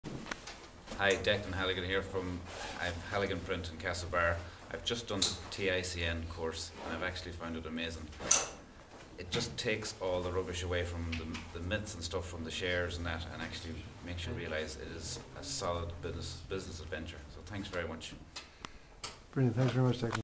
TICN Club Mayo Club Member
Declan  gives us feedback on the Edcuation Modules